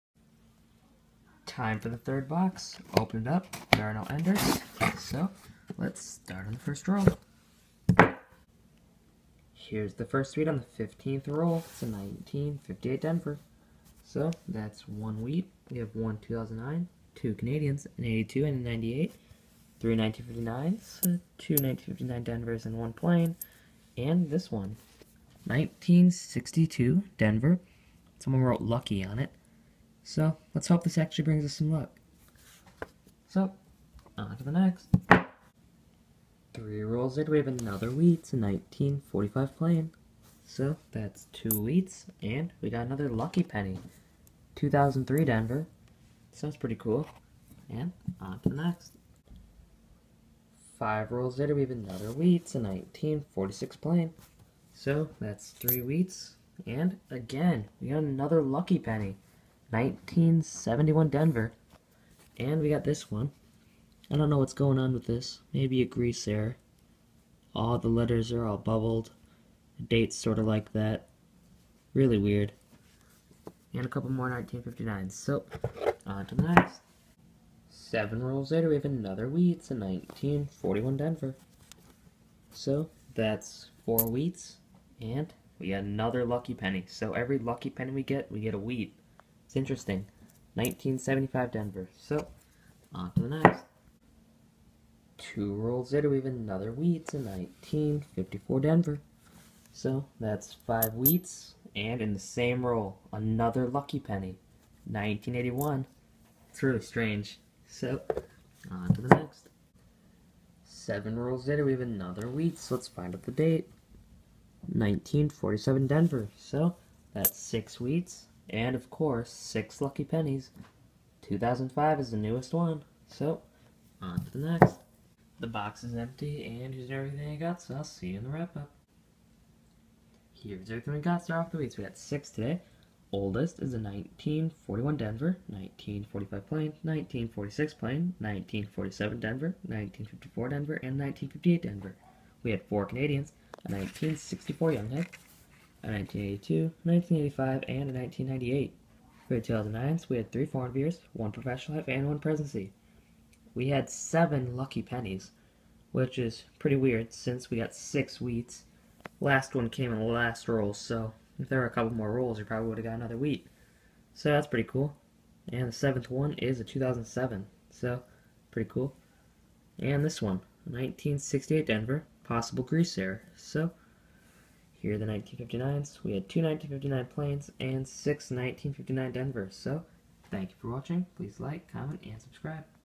Are these pennies actually lucky? -Coin Roll Hunting-
In this video, I will be searching through $25 in pennies looking for Indians, wheats, errors, foreigns, and other cool coins.